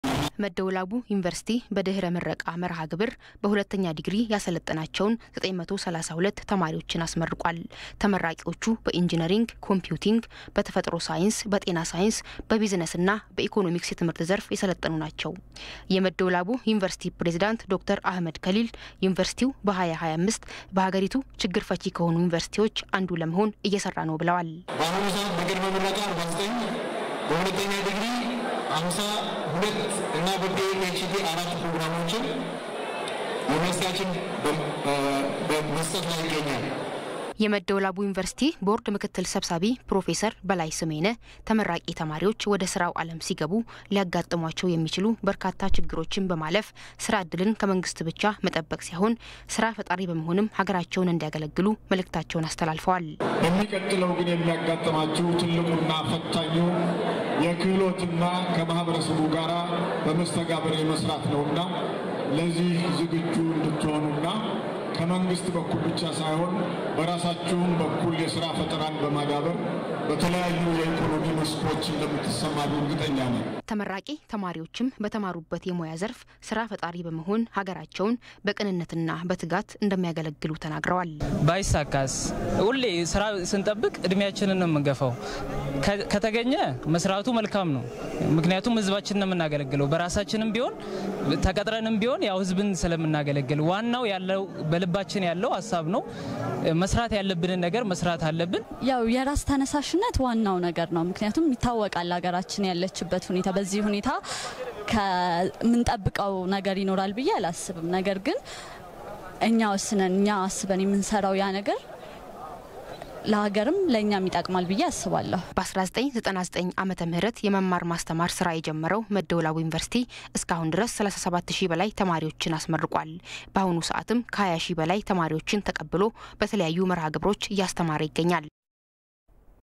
የመደ ወላቡ ዩኒቨርስቲ ምርቃት Etv | Ethiopia | News
የመደ ወላቡ ዩኒቨርስቲ ለ14ኛ ዙር ያስተማራቸውን 932 ተማሪዎችን በመጀመሪያ እና በሁለተኛ ዲግሪ አስመረቀ፡፡